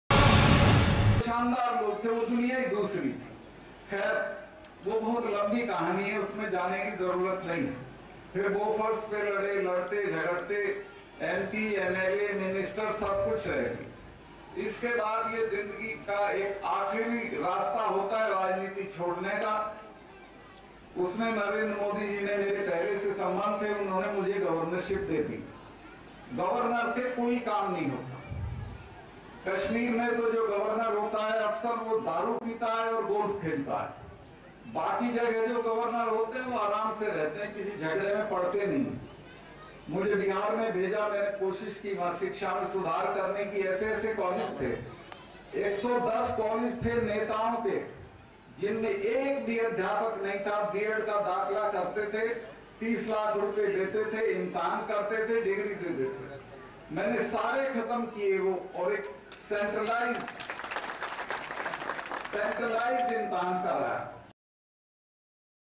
Jammu Kashmir | जम्मू-कश्मीर के राज्यपाल के पास दारू पीने के अलावा कोई काम नहींः सत्यपाल मलिक
जम्मू-कश्मीर के राज्यपाल के पास दारू पीने के अलावा कोई काम नहींः सत्यपाल मलिक

गोवा के मौजूदा और जम्मू-कश्मीर के पूर्व राज्यपाल सत्यपाल मलिक ने एक बयान में कहा है कि राज्यपाल के पास कोई काम नहीं होता है। जब जम्मू-कश्मीर से अनुच्छेद 370 हटाया गया उस वक्त वहां के राज्यपाल रहे सत्यपाल मलिक ने दावा किया कि कश्मीर में जो राज्यपाल होता है अकसर वो दारू पीता है और गोल्फ खेलता है।